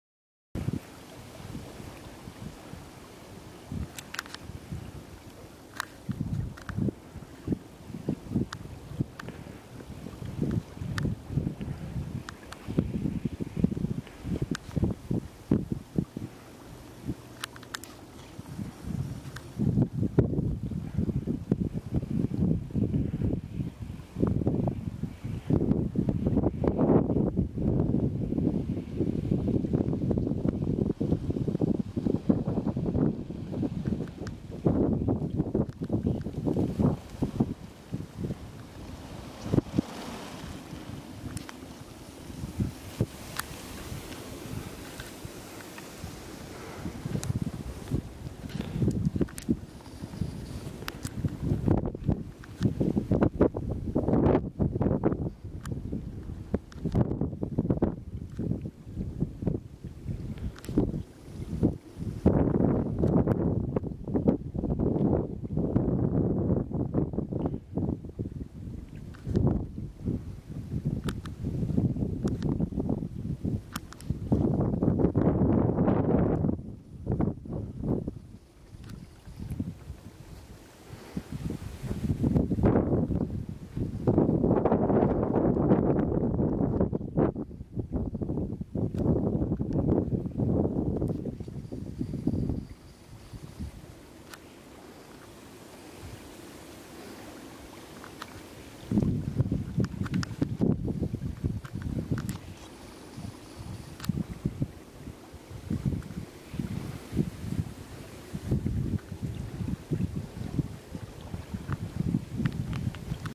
Vogelschwarm in der Goitzsche bei Bitterfeld
Goitzsche, Nähe Bärenhof

Februar 2015

Fischadlerhorst im Hintergrund